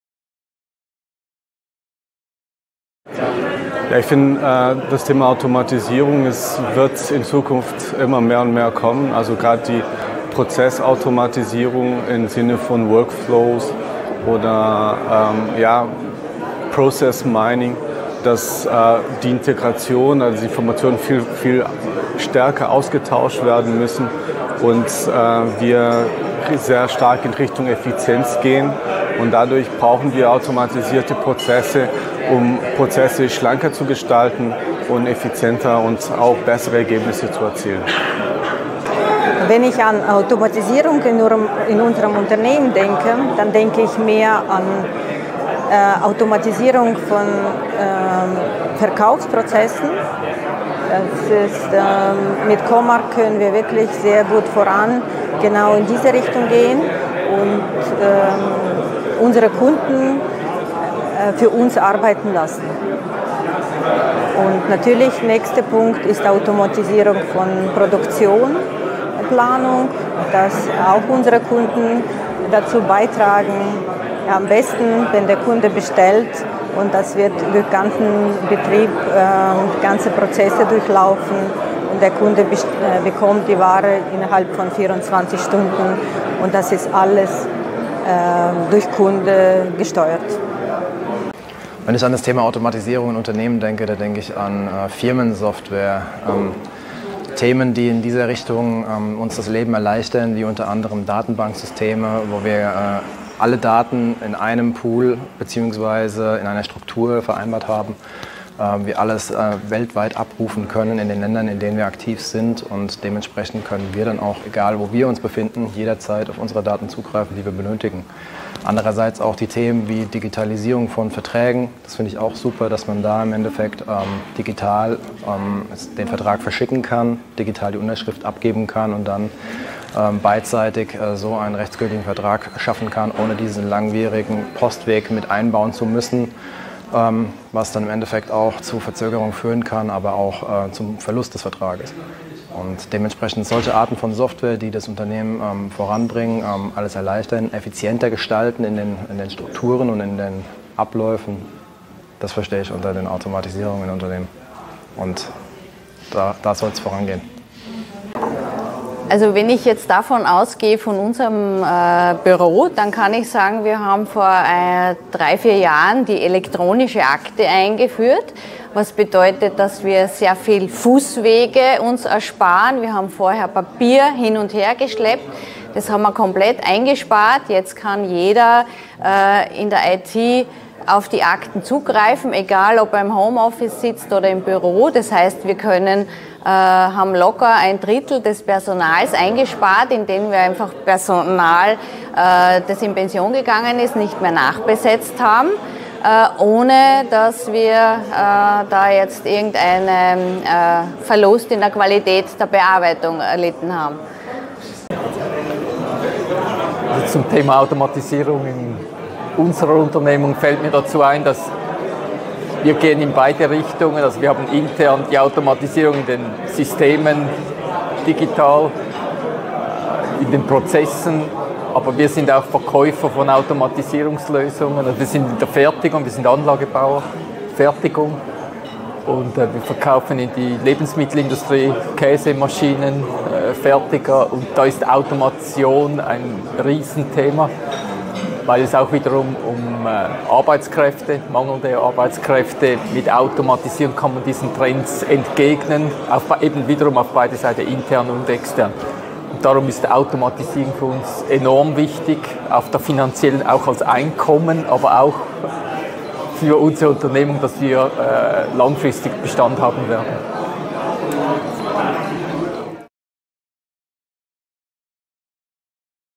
Meinungen zu Automatisierungstrends 2022 auf den Comarch Kundentagen in Berlin
Wir haben unsere Kunden gefragt, was Ihnen zuerst in den Sinn kommt, wenn Sie an Automatisierung in Ihrem Unternehmen denken. Hören Sie selbst!

Wie Automatisierung mit Intelligenter Prozessautomation (IPA), Robotic Process Automation (RPA) und Künstlicher Intelligenz (KI) in Produktion und Handel gelingt, zeigt die aktuelle IDC-Studie